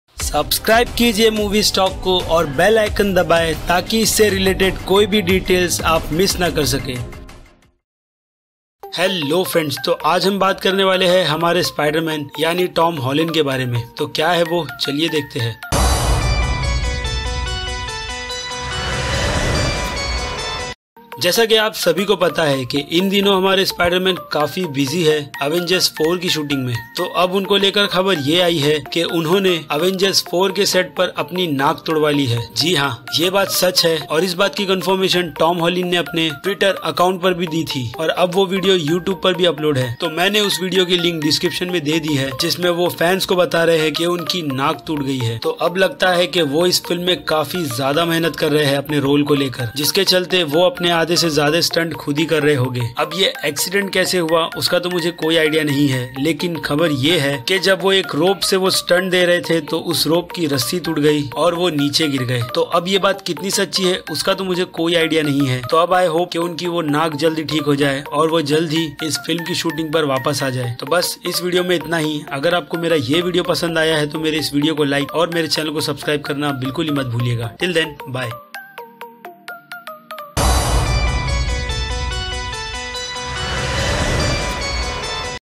Spider-Man Breaks His Nose On AVENGERS 4 Set [Explain In Hindi]
Hello friends aaj hum baat karne wale hai spider-man ke bare me to kya hai woh chaliye dekhte hai